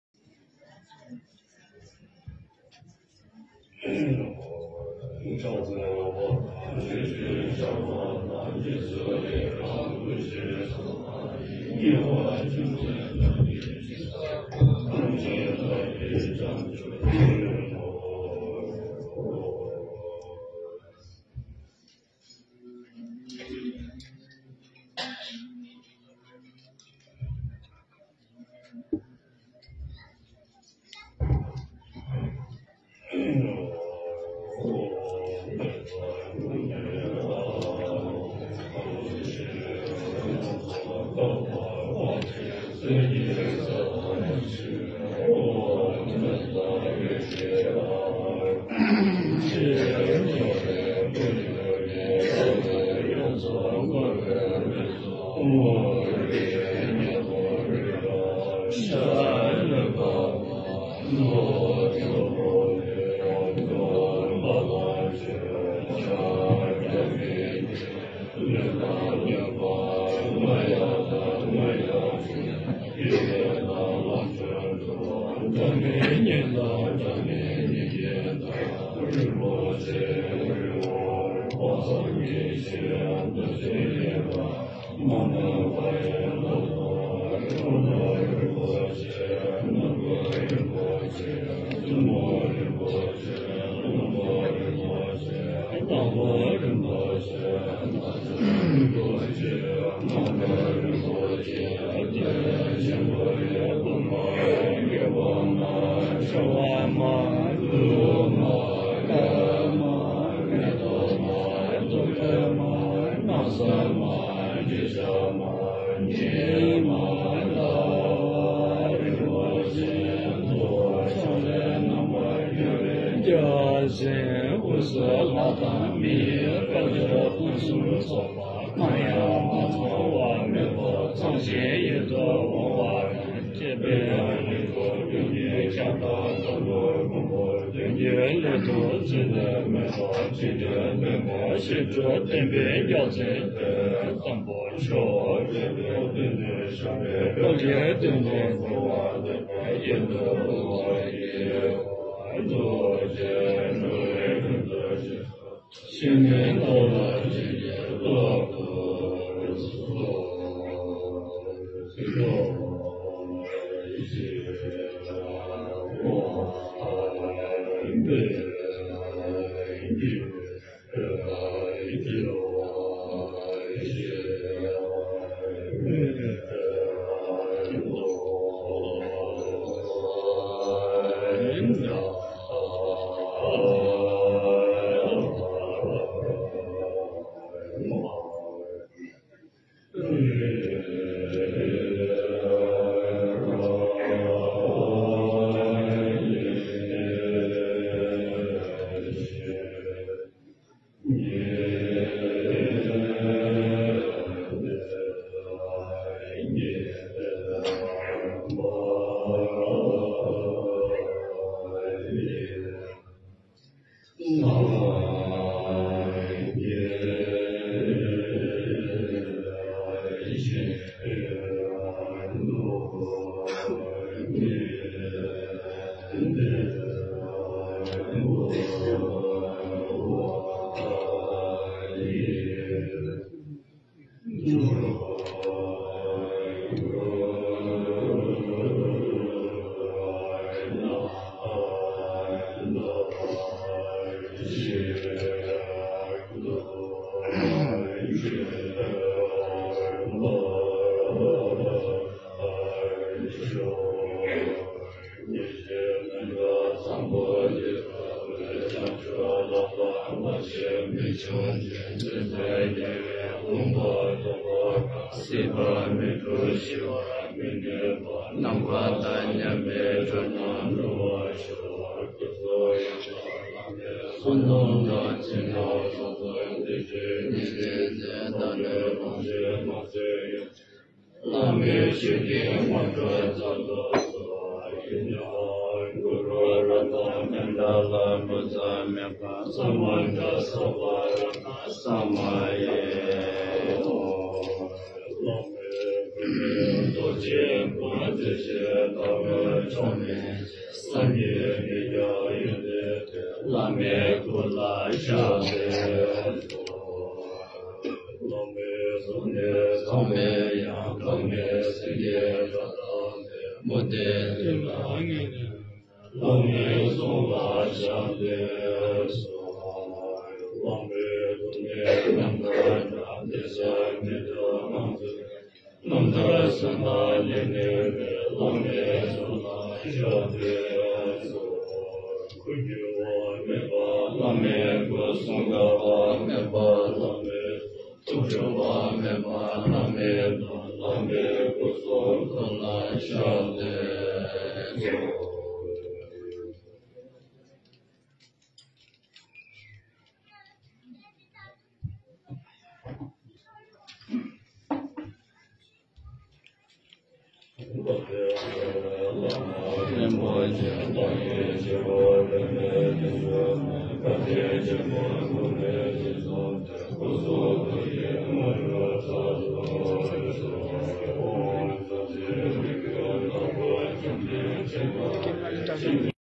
Namkhai Nyingpo Rinpoche is offering Mandala to Reincarnation of Tsetrul Rinpoche.
video courtesy  :  Karma Bodh